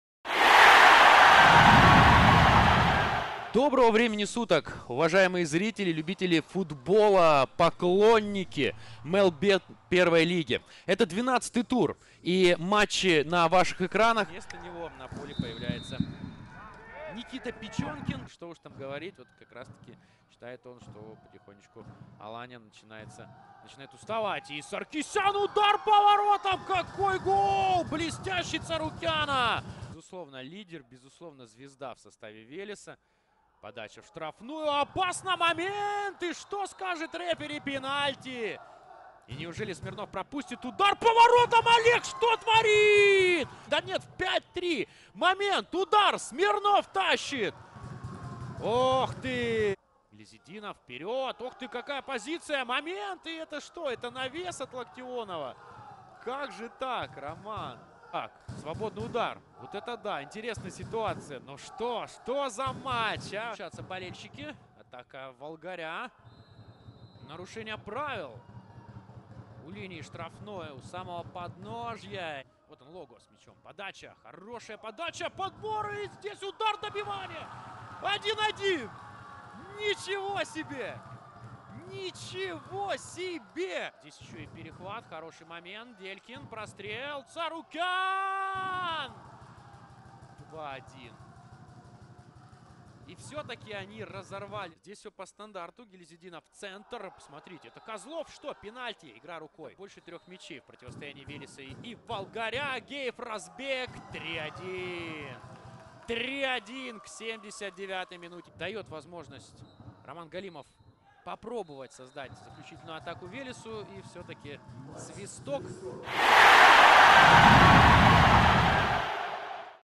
120 секунд о матче «Велес» — «Волгарь» | 12 тур МЕЛБЕТ-Первой лиги
Лучшие моменты из игры «Велеса» (Москва) и «Волгаря» (Астрахань) 1 октября 2022.